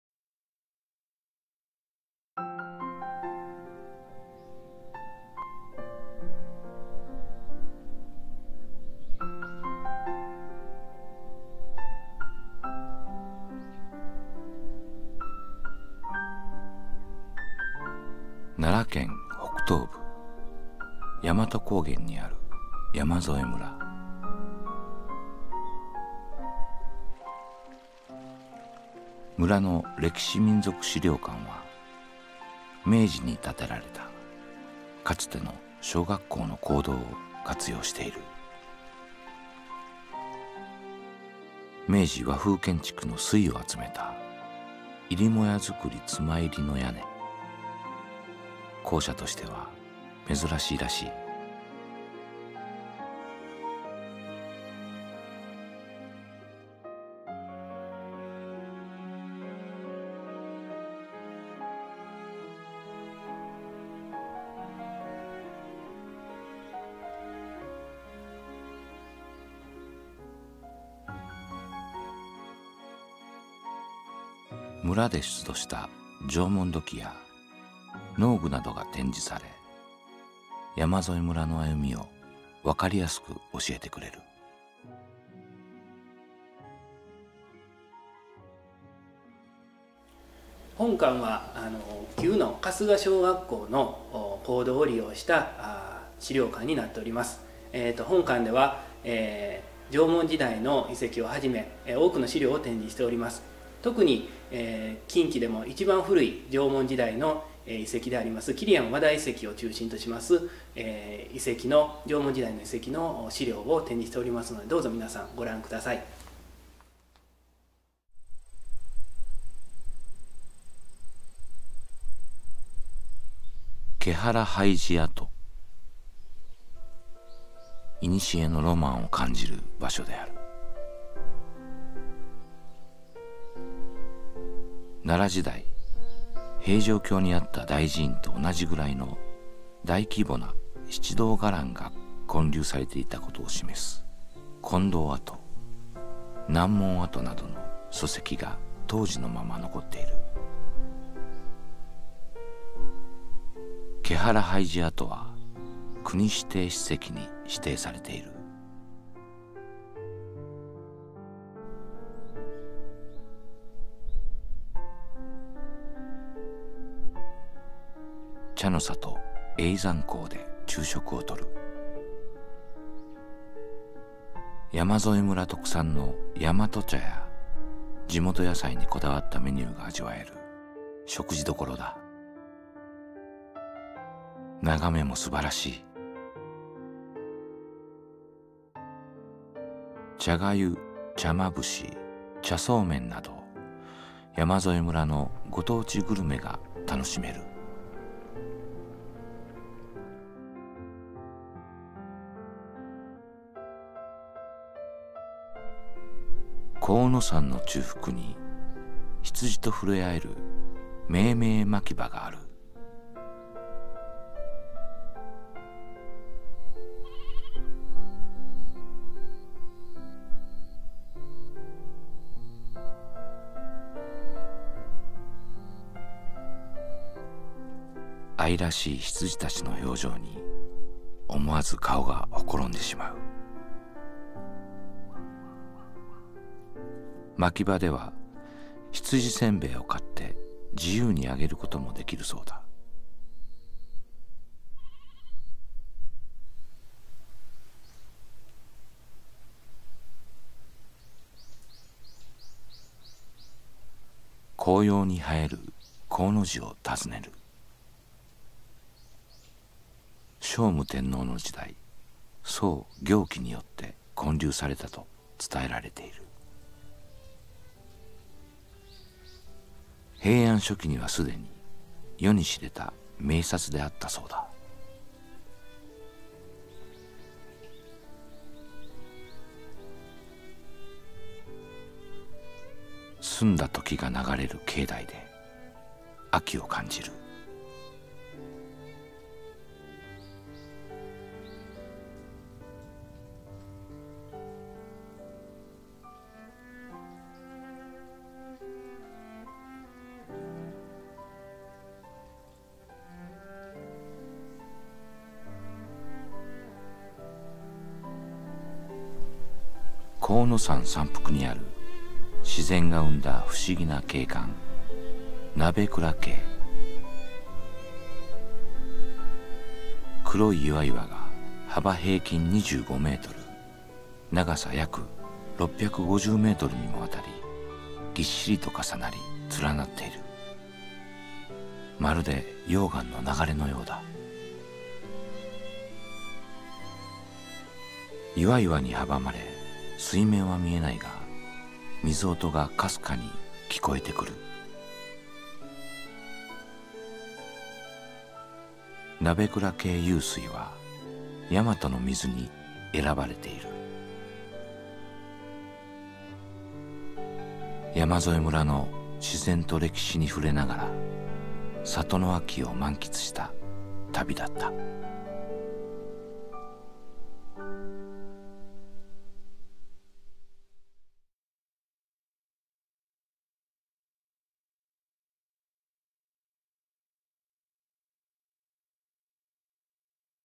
旅の星 Tabinohoshi 「山添村～ふるさとの秋景」 Yamazoe, Japan vol.21
☆Narration: 日本語
★Please select English (CC) for English captions.

奈良県　山添村は、
大和高原の一部。
のどかな自然と歴史に
ふれあえる里だ。
今回は、秋の空気を感じながら、
村の名所名跡を訪ねた。

Yamazoe Village – Hometown Autumn Scenery
Yamazoe Village, Yamabe District, Nara Prefecture

This film showed you the sites and sounds of autumn in Yamazoe,
a village in the Yamato Highlands of Nara Prefecture
with a picturesque landscape and enlightening history.

☆詳細ＬＩＮＫ☆

Chie Ito,Kaai Hanashita